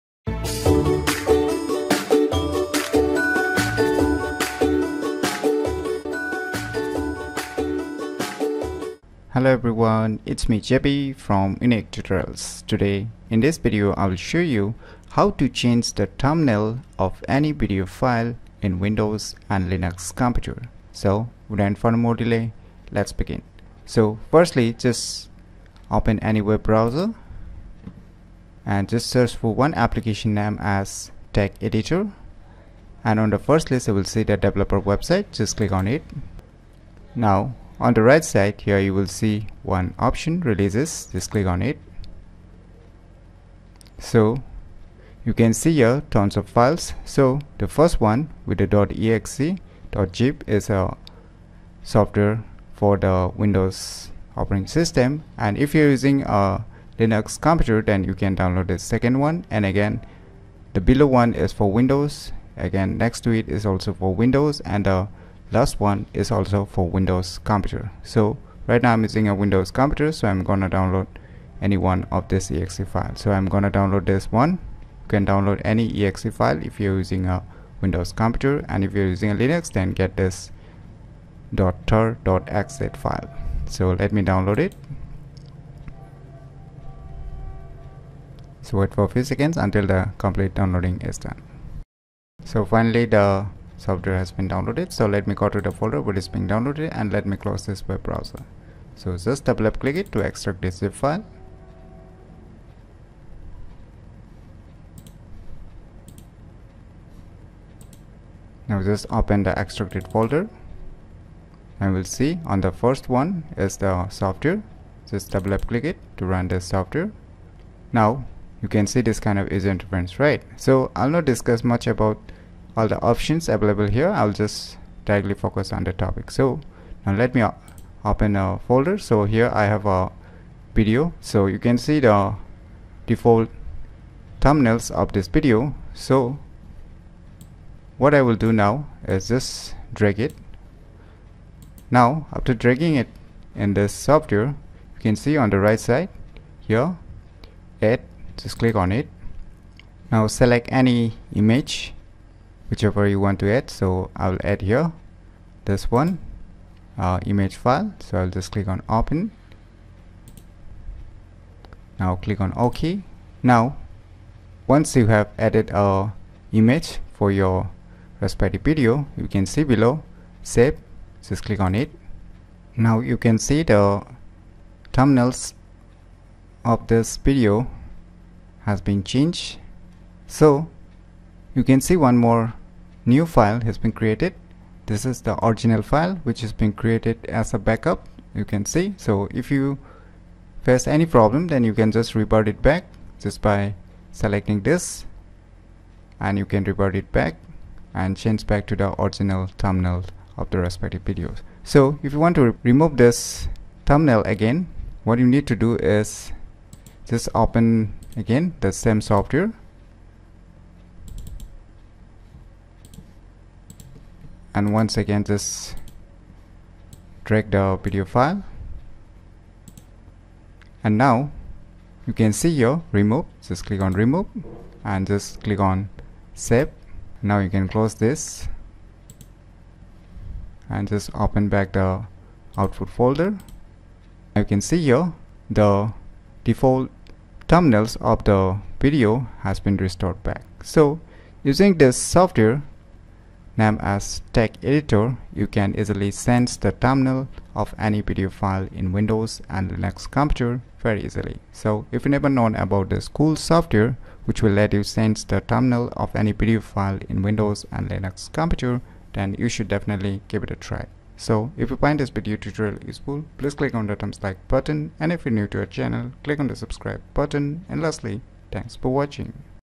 How to change the Thumbnail of any Video file in Windows and Linux computer ?
This video tutorial is all about How to change the Thumbnail of any Video file in Windows and Linux computer.